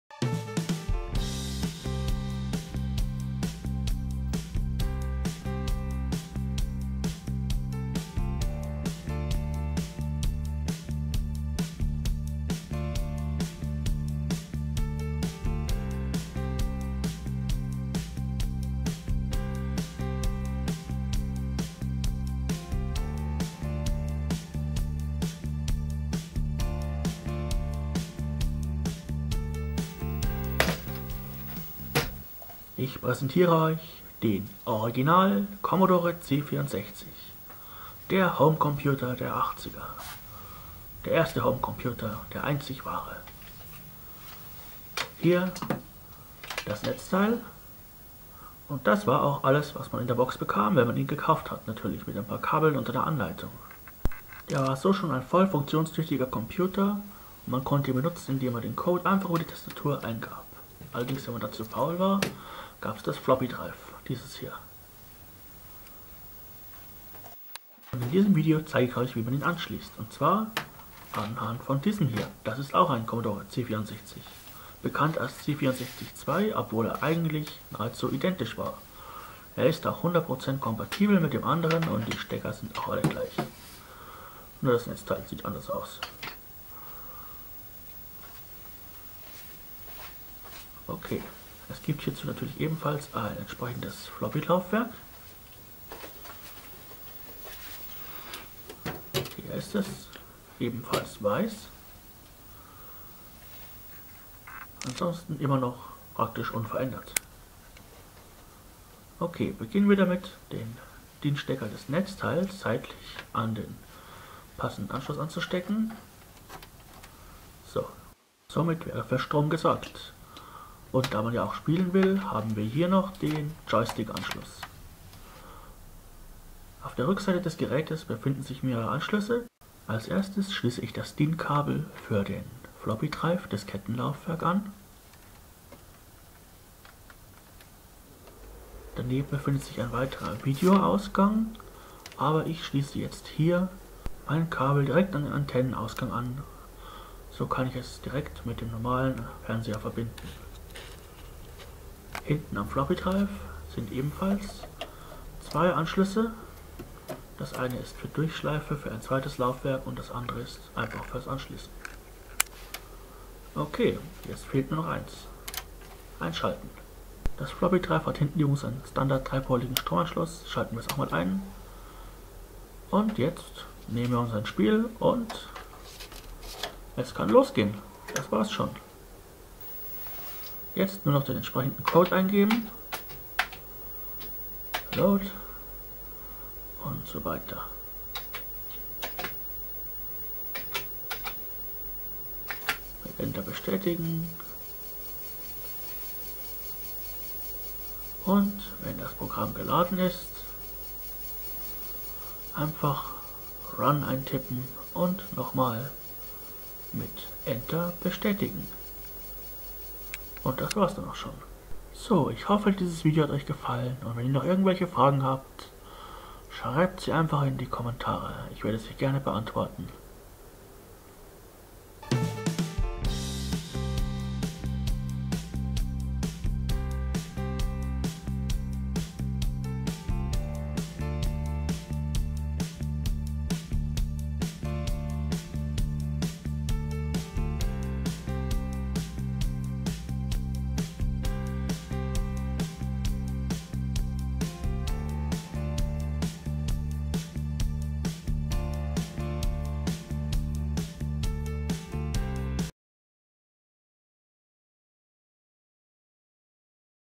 Commodore c64 für Einsteiger ( Anleitung )
Part1 der C64 Serie: Anschließen und Verwenden